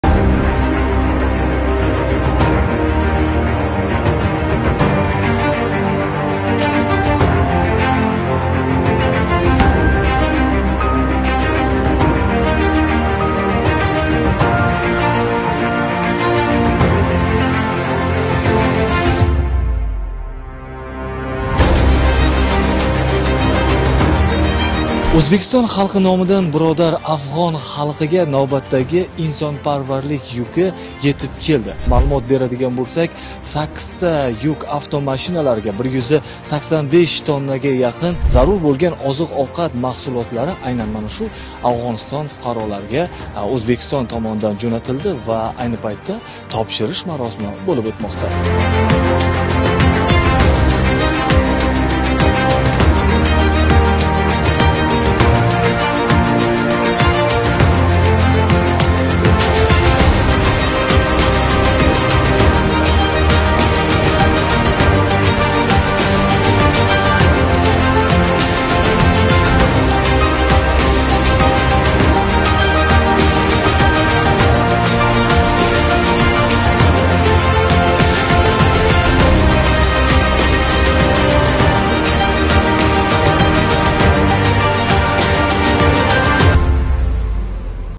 Ўзбекистон халқи номидан биродар афғон халқига навбатдаги инсонпарварлик ёрдами етеазилди
Афғонистонлик расмийлар Муборак Рамазон ойида кўрсатилаётган бу каби амалий ёрдам учун Ўзбекистон халқига ўз миннатдорчилигини билдирди.

Инсонпарварлик ёрдами таркибидан 48 тонна ун, 48 тонна буғдой, 15,7 тонна ўсимлик ёғи, 10 минг 20 дона консерваланган гўшт маҳсулотлари (тушёнка), 22 тонна макарон, 24 тонна шакар ҳамда 100 минг дона тез тайёрланадиган лапша (Hot lanch) ўрин олган.